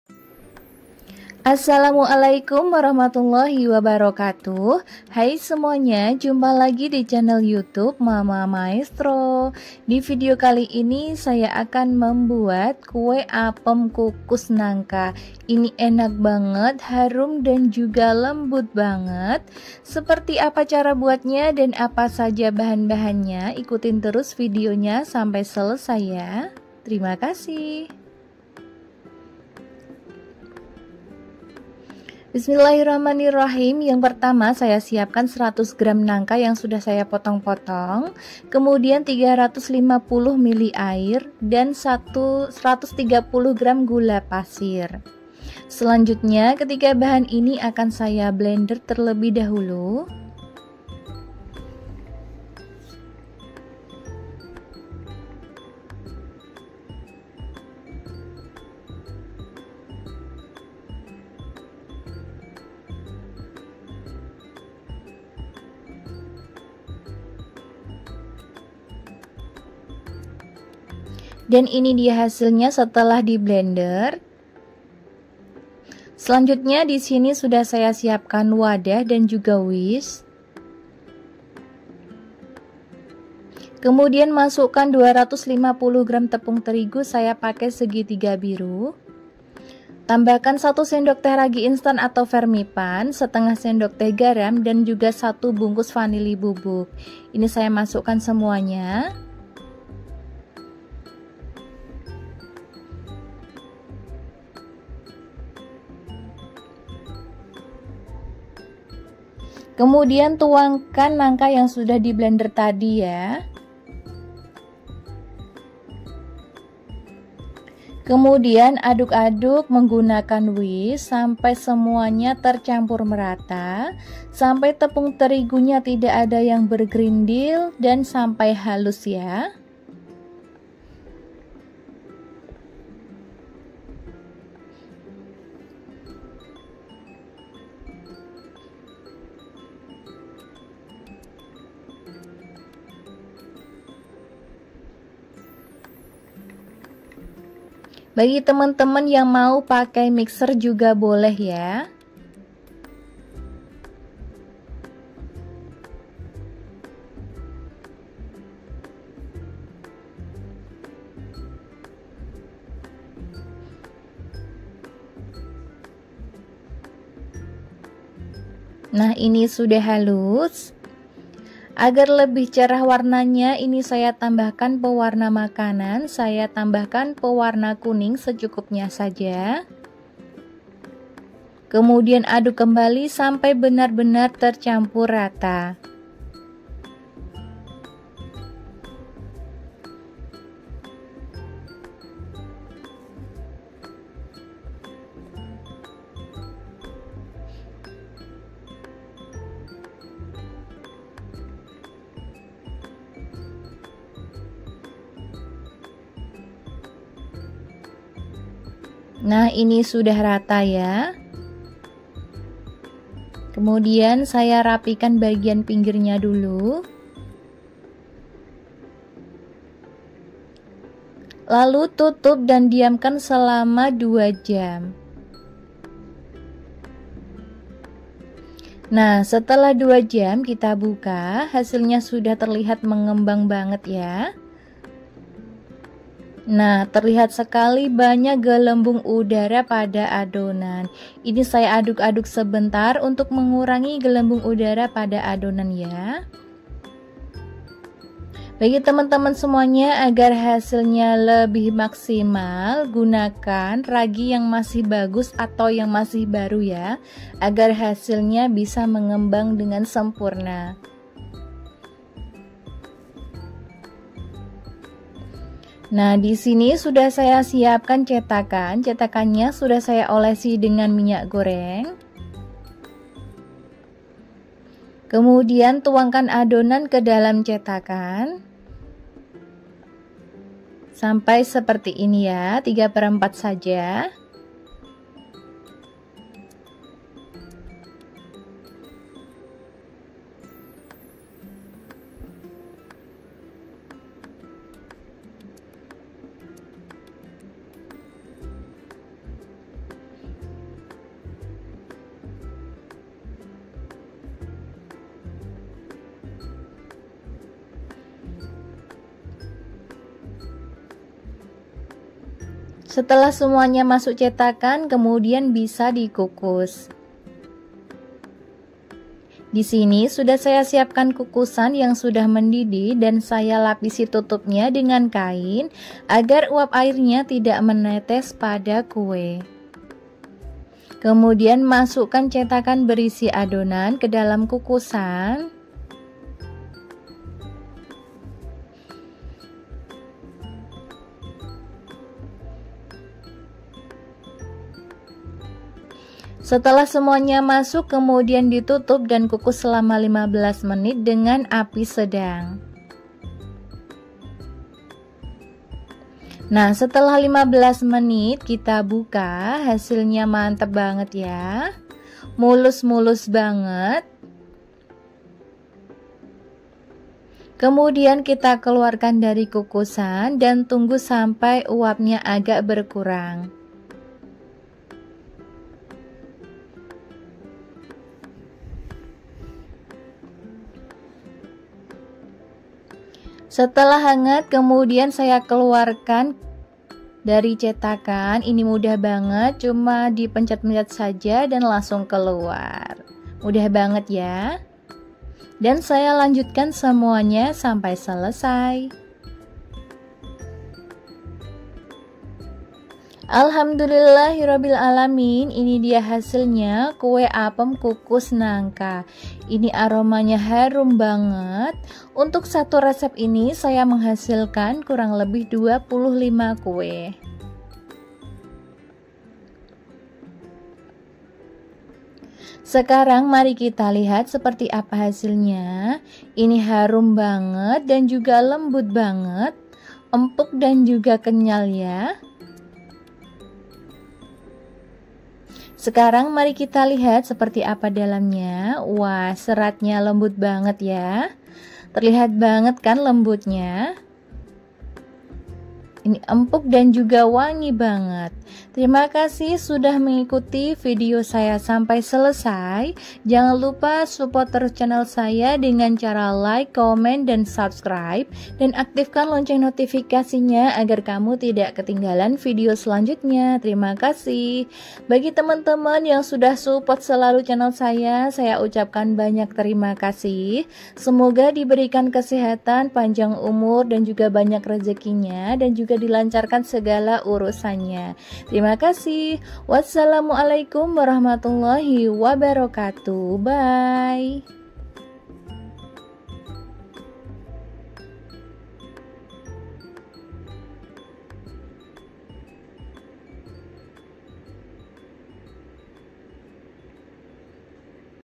RESEP APEM KUKUS NANGKA || HARUM DAN LEMBUT BANGEET..
Assalamualaikum temen2...
di video kali ini saya membuat apem kukus nangka.
aromanya harum, dan kuenya lembut kenyal.

RESEP APEM KUKUS NANGKA

100 gr nangka
350 ml air
130 gr gula

250 gr tepung terigu pro sedang
1 sdt ragi instan / fermipan
1/2 sdt garam
vanili bubuk

✴️diamkan donan selama 2 jam
✴️kukus selama 15 menit dg api sedang
✴️1 resep jadi 25 kue.